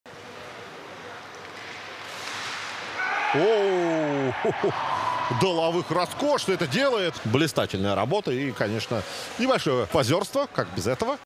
Забросил красивый гол и отобрал «хлеб» у судей!
Нападающий «Красноярских Рысей» Иван Доловых забросил очень красивый буллит в ворота «Кузнецких Медведей»